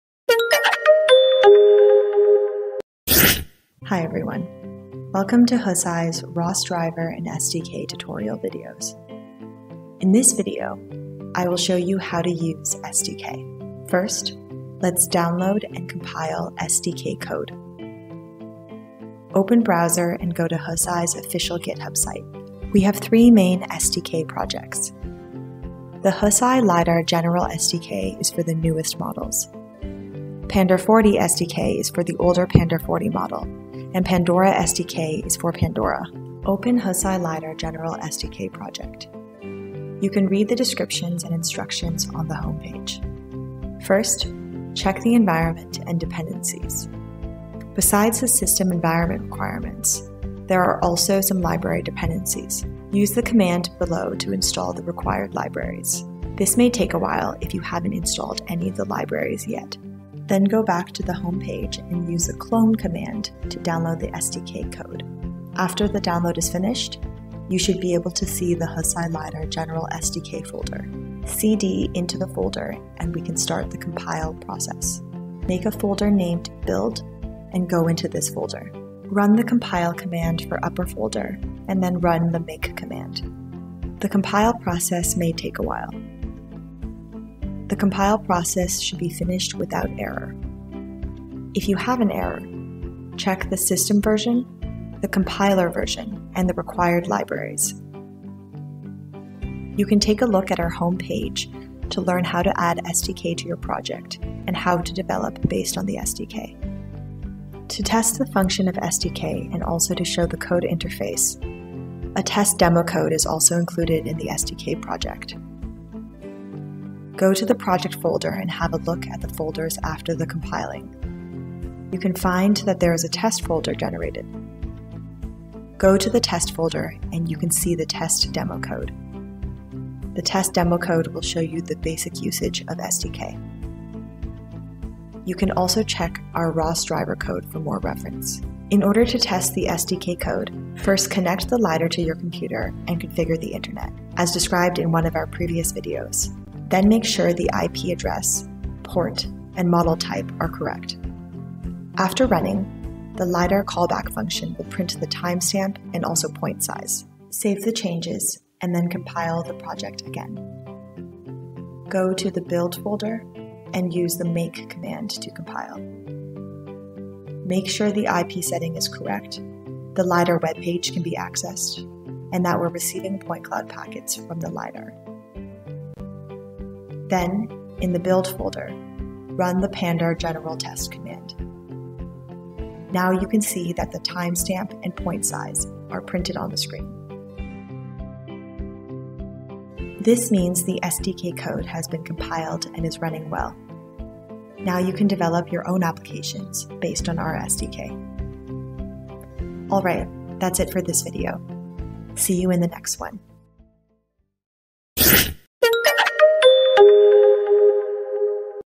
Hesai Lidar Tutorial #4: Compile and Test SDK
Welcome to Hesai's Lidar Tutorial video series. These videos will cover commonly asked questions and other relevant topics to help users successfully set up and use their Hesai Lidar.